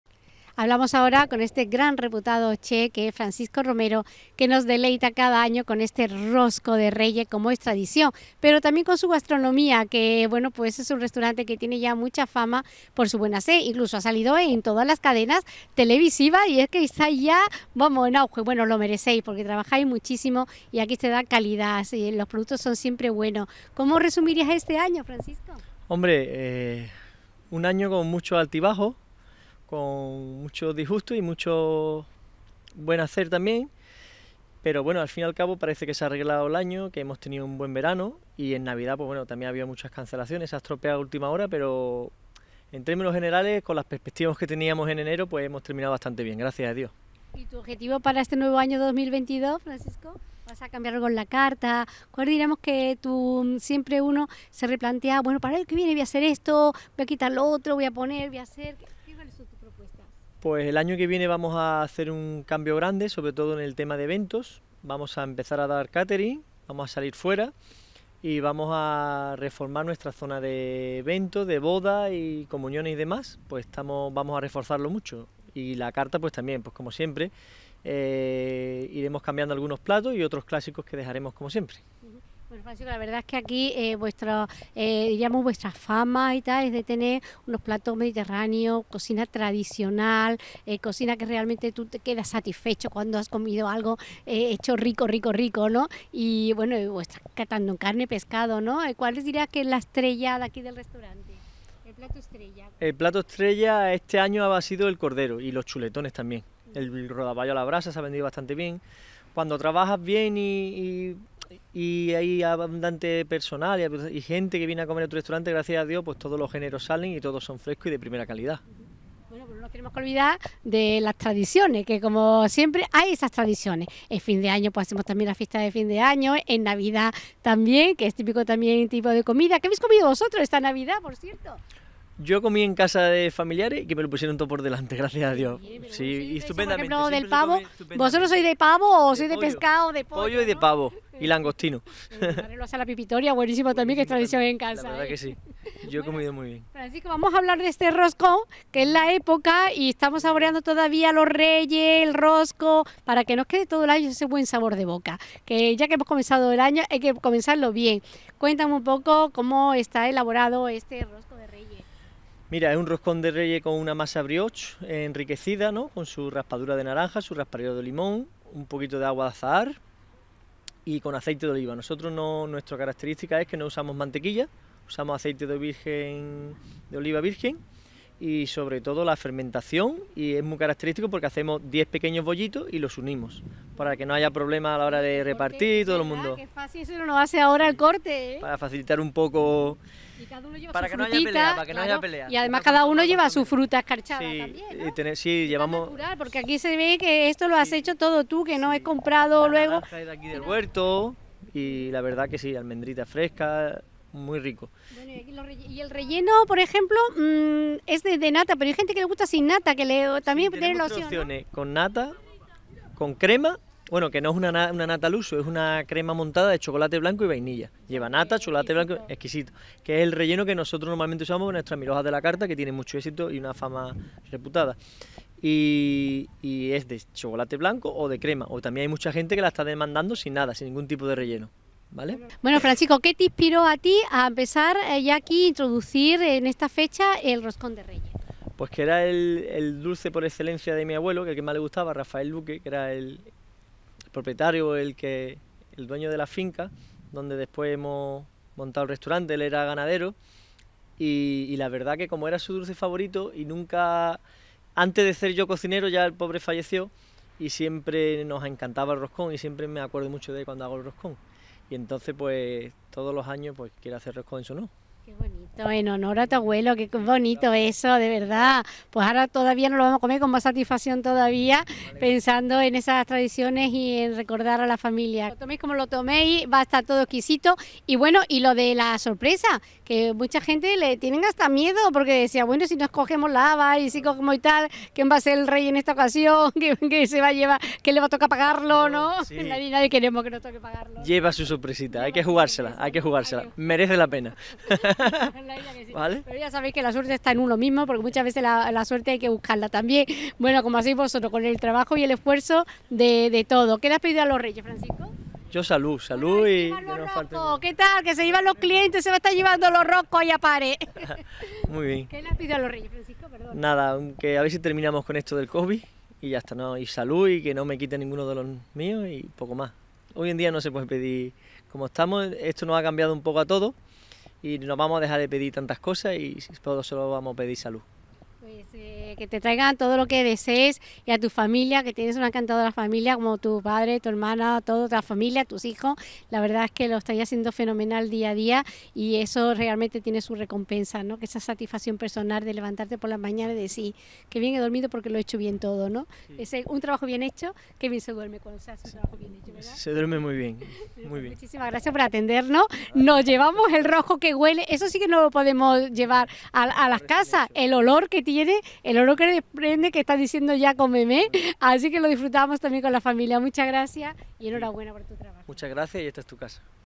Roscón de Reyes Entrevista de Helena Olaya a Francis Romero Chef en Restaurante el Gamona San Pedro
Roscón de Reyes-Entrevista de Helena Olaya a Francis Romero-Chef en Restaurante el Gamona-San Pedro Alcántara